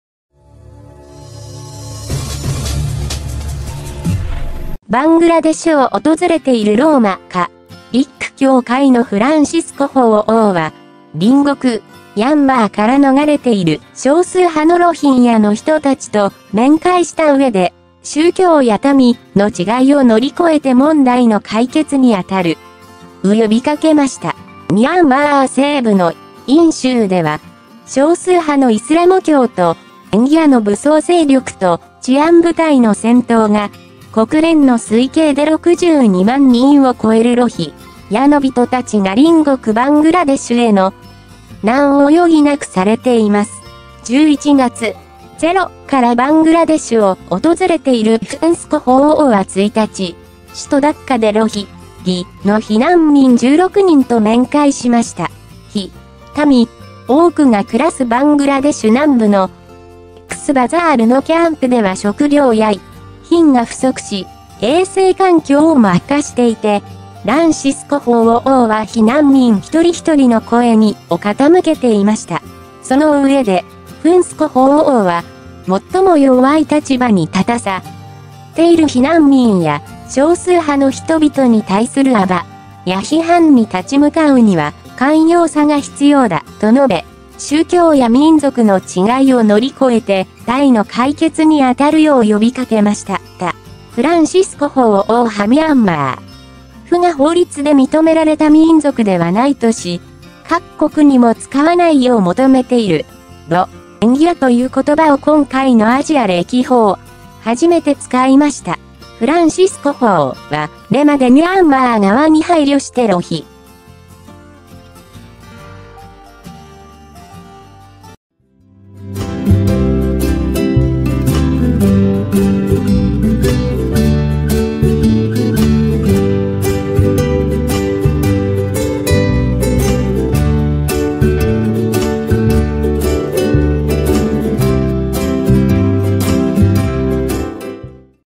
ローマ法王がロヒンギャと面会 問題解決呼びかけ
バングラデシュを訪れているローマ・カ
リック教会のフランシスコ法王は、隣国
ャンマーから逃れている少数派のロヒン
ャの人たちと面会したうえで、宗教や民
の違いを乗り越えて問題の解決に当たる. う呼びかけました。ミャンマー西部の。
イン州では、少数派のイスラム教徒、。
ンギャの武装勢力と治安部隊の戦闘が。
、国連の推計で６２万人を超えるロヒ。
ャの人たちが隣国バングラデシュへの. 難を余儀なくされています。１１月。０
からバングラデシュを訪れているフ。ン
スコ法王は１日、首都ダッカでロヒ。ギ
の避難民１６人と面会しました。避。民
多くが暮らすバングラデシュ南部の. ックスバザールのキャンプでは食料や医
品が不足し、衛生環境も悪化していて、
ランシスコ法王は避難民一人一人の声に
を傾けていました。そのうえで、フ。ン
スコ法王は「最も弱い立場に立たさ. ている避難民や少数派の人々に対する暴
や批判に立ち向かうには寛容さが必要だ
と述べ、宗教や民族の違いを乗り越えて
題の解決に当たるよう呼びかけました。
た、フランシスコ法王はミャンマー. 府が法律で認められた民族ではないとし
、各国にも使わないよう求めている「ロ
ンギャ」という言葉を今回のアジア歴訪
初めて使いました。フランシスコ法。は
れまでミャンマー側に配慮してロヒ.